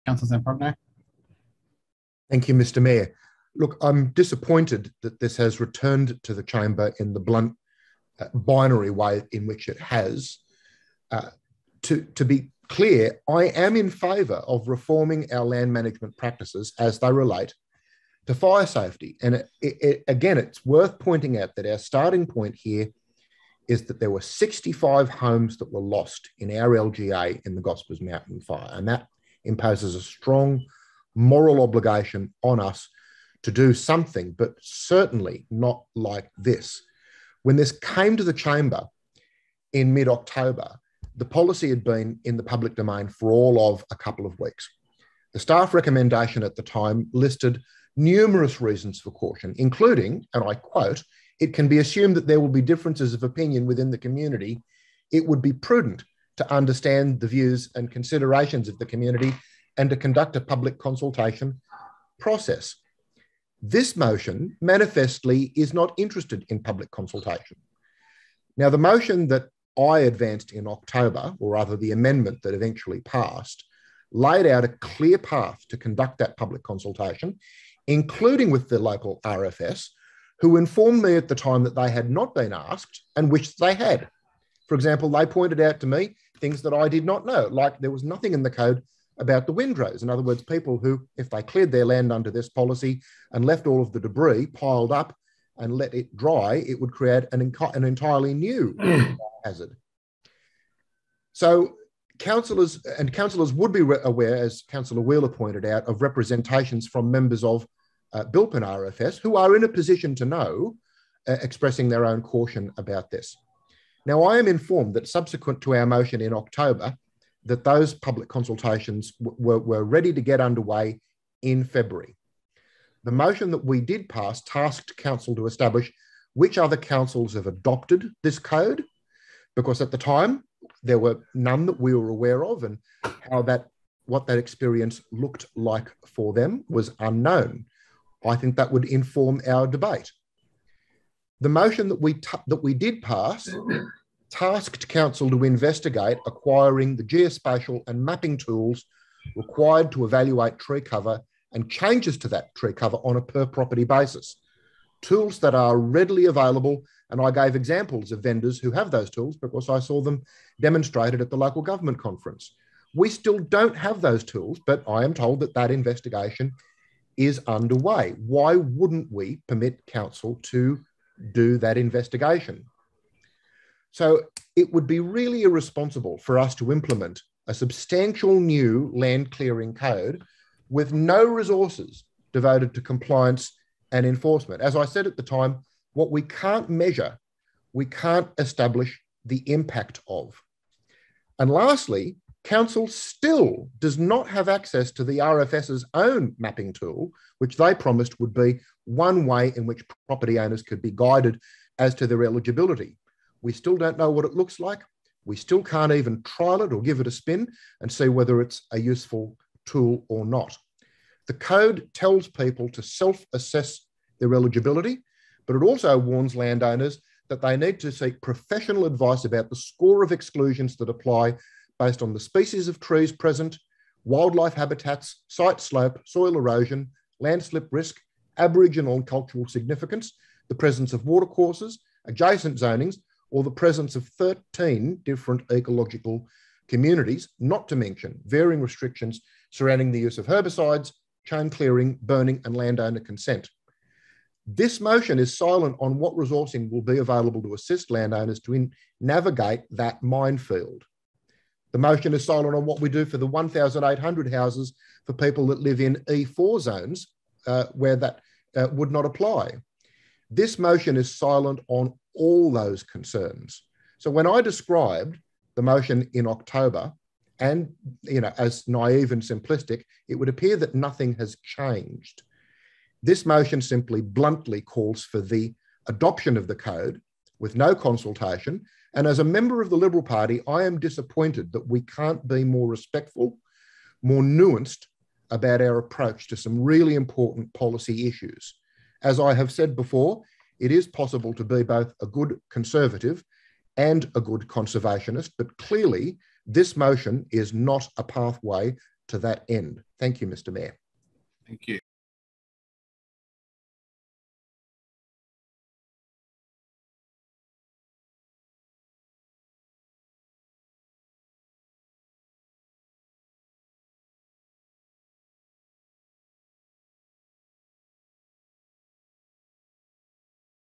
Council adopts the Rural Boundary Clearing Code (Jan 2022 redux)
Last October some of my Liberal colleagues tried to ram through the adoption of the "Rural Boundary Clearing Code", which would allow the clear-felling of up to 15,800 hectares of rural land where landowners assert such clearing is needed to improve fire safety.

I think that Council has a moral obligation to look at this area, given the loss of 65 homes in the Hawkesbury in the fires.

But balancing community safety with protection of the environment is challenging. It takes leadership, and nuancing the various issues and views, not ramming something through without consideration of the consequences.

That attempt last year failed.
Firstly, we had conducted zero formal consultation with our local RFS.
Secondly, it was clear that we needed to resource Council to measure the effects of this policy with geospatial mapping tools, provide guidance to landowners about the torturous eligibility criteria, and ensure we could conduct basic compliance and enforcement. We had no knowledge of a single other Council taking up the voluntary adoption of this code and what that experience looked like for them, and many local experts told me that they were either opposed, or took a "yes, but" approach contingent on these basic precautions.

I advanced an alternative motion to get Council to conduct that consultation and resourcing, and I got it passed. The report would have come back to Council this year.

However, the complexion of the Council chamber has changed, and on Tuesday my Liberal colleagues again tried to ram summary adoption of the code through. This time, they had the numbers.
So that's it. No consultation. No resourcing for partnering with landowners. No resources for mapping, compliance or enforcement.
Oh, and no takebacks. Once we're in, we're in for good.

I think of this as a massive slap in the face for our local RFS, who are free to be for or against this, but who would have at least been accorded the courtesy of being *asked*. So much for the oft-claimed respect for our RFS.
I've always claimed that it's possible to be a good Conservative, and a good Conservationist. It's a pity that this looks nothing like that.
Environmentally conscious voters in the Hawkesbury, and if I might suggest, across the seat of Macquarie ought to keep their own counsel about whether this looks like the kind of representation they want.
In favour of the summary adoption of the code was:
Mayor Conolly, Clr. Richards, Clr. Reardon, Clr. Veigel, Clr. Sheather, Clr. Dogramaci.
Passage was ensured with the casting vote of the Mayor after a 6-all deadlock.